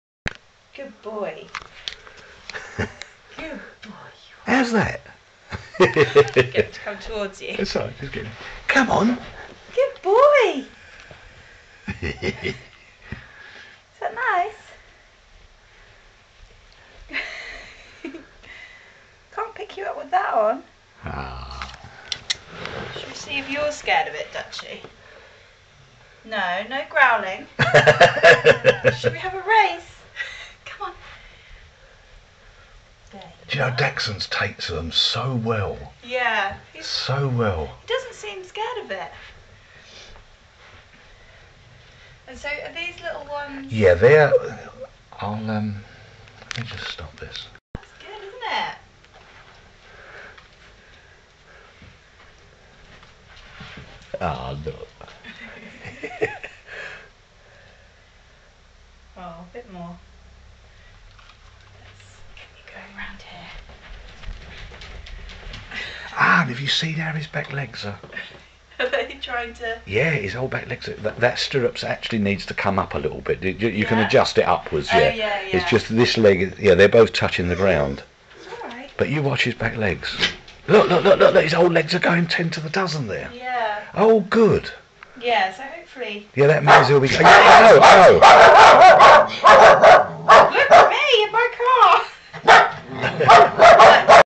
Napoleon the Dachshund's first go in his wheels, instant success !
This is Napoleon a miniature Dachshund with the usual disc problems taking to his new wheels immediately